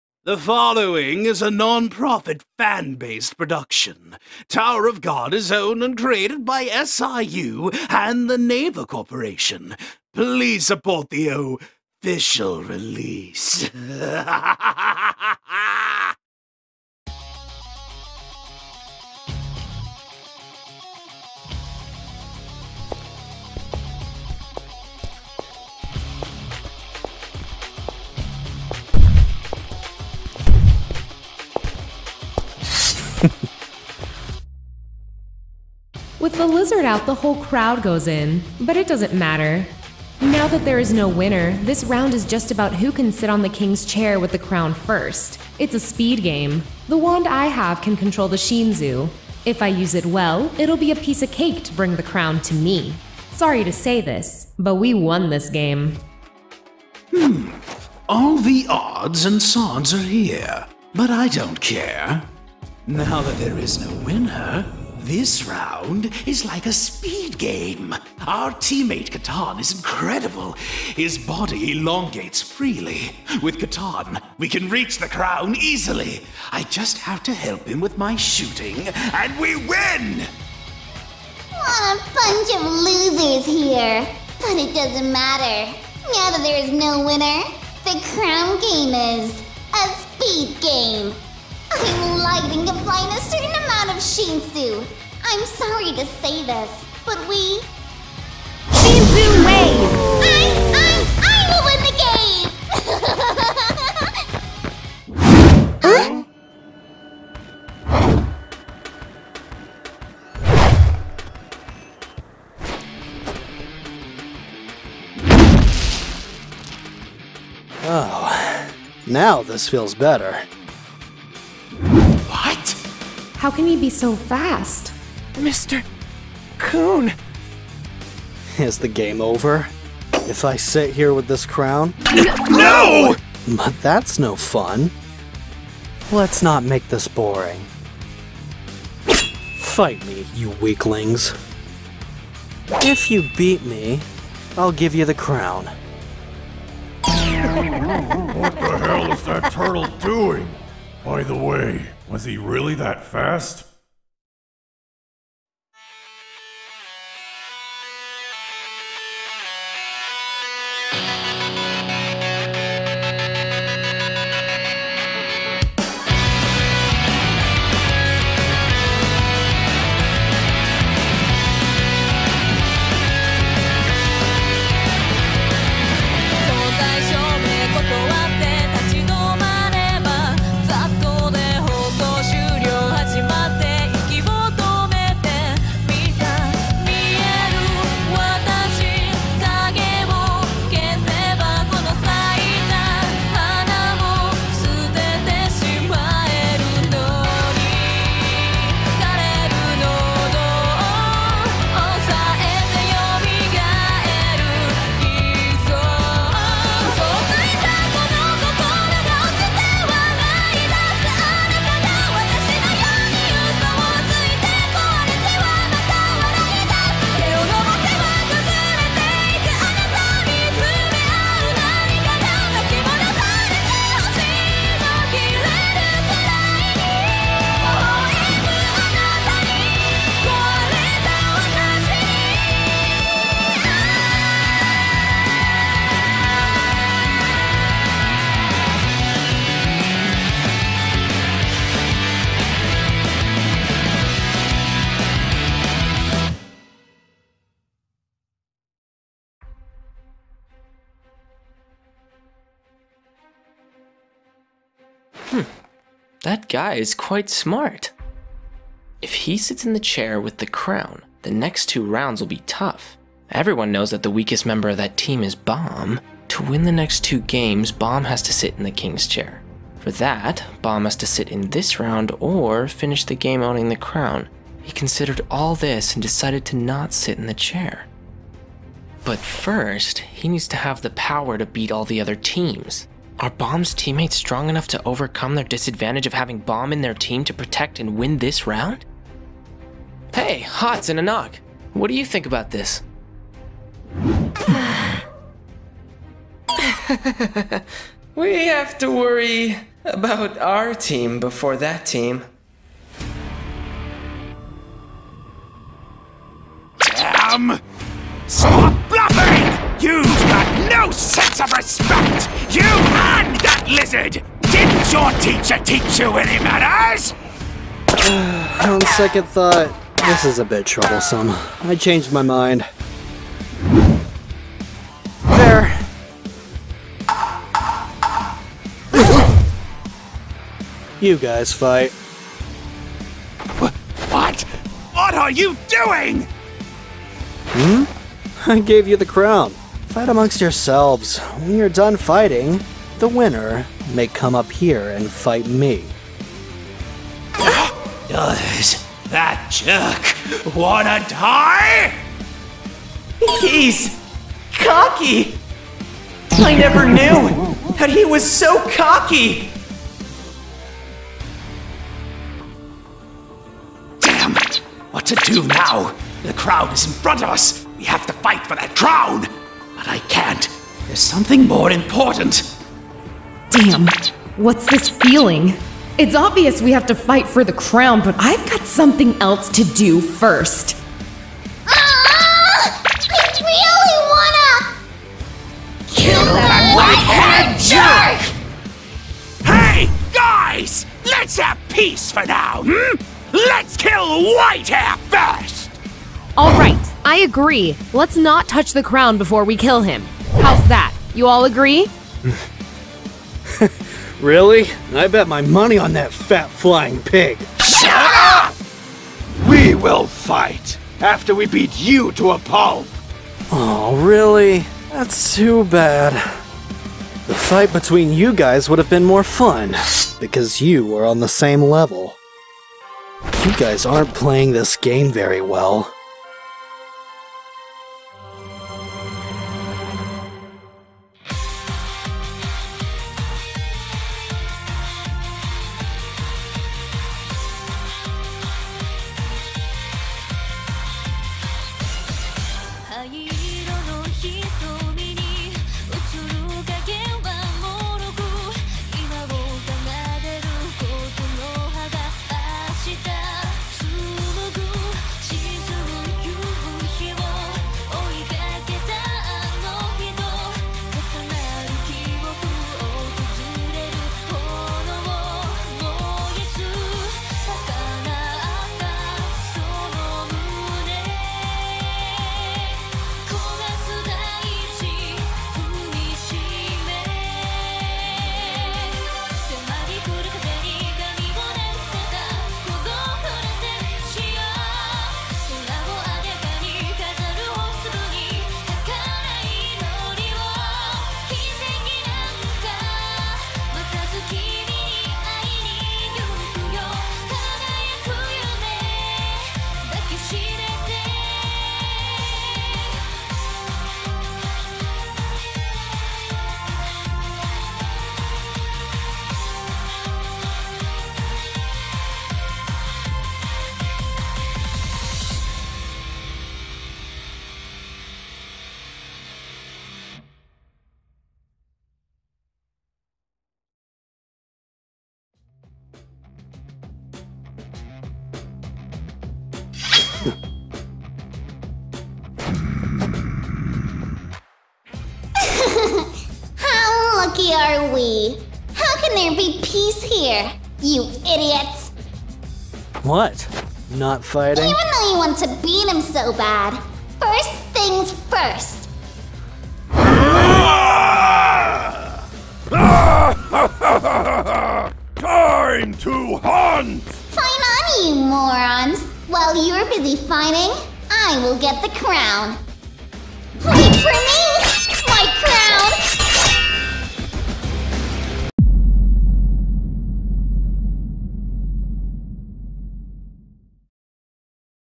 Tower of God Dub: Ep. 21 - Round 3
Tower of God Comic Dub: Episode 21! Round 3 begins at last, with Khun's team challenging three other teams who are also competing for the crown. While each of the team leaders plan out their strategy, a surprising flash of speed catches everyone's eye...

Please support the official release!

CAST

MUSIC